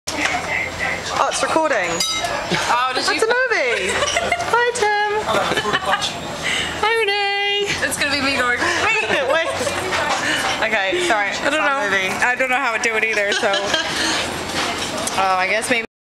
Restaurant
Video taken by mistake.  A friend thought she was taking a pic.